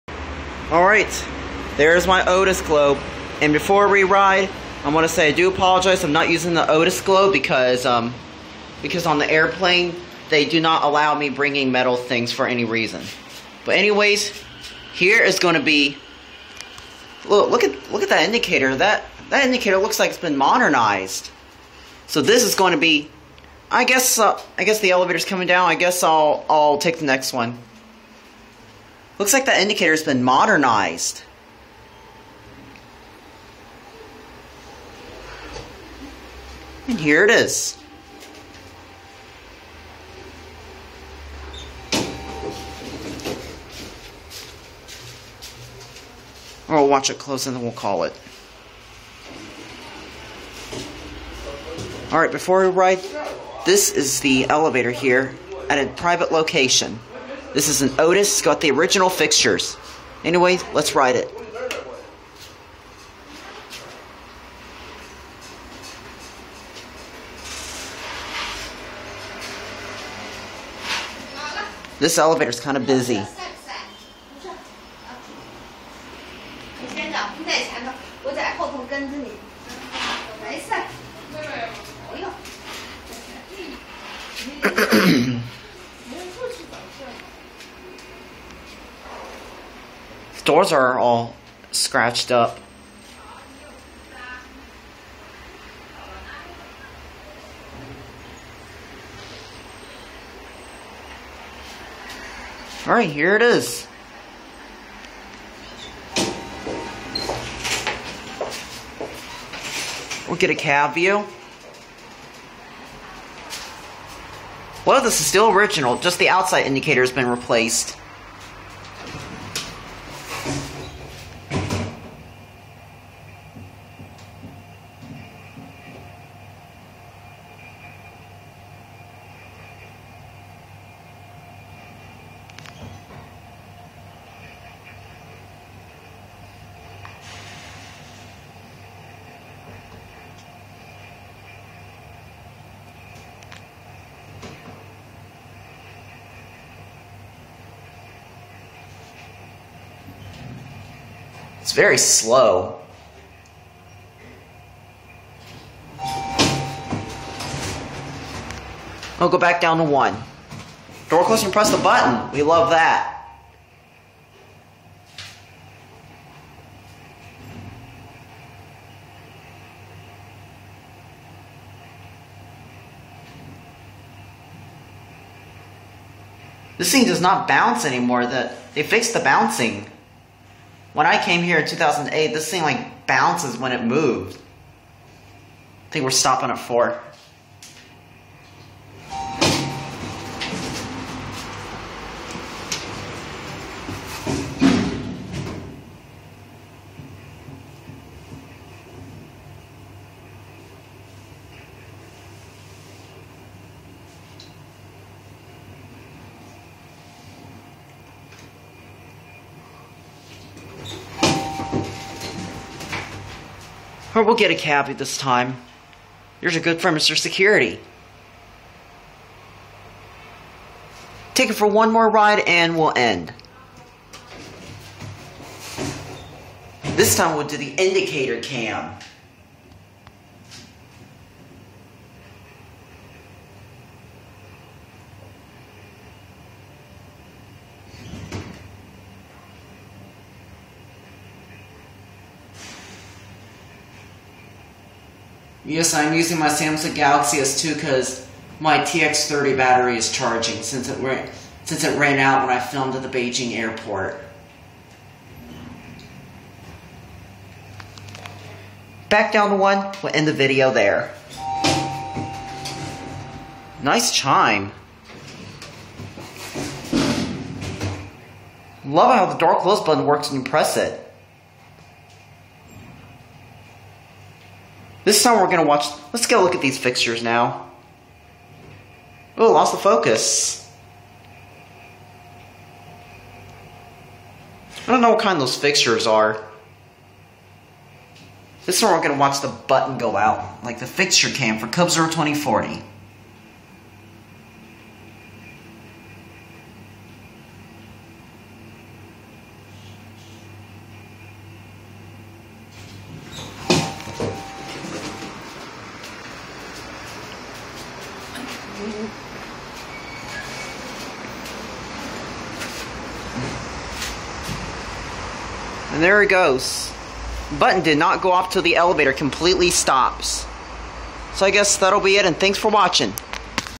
XiZi Otis Traction Elevator at my aunt's apartment
Filmed on July 5, 2014 (Asian time): I took this on my S2 because my TX30 was charging battery and I do have a video taken on that camera as well.

The name of this location will not be given for private reasons.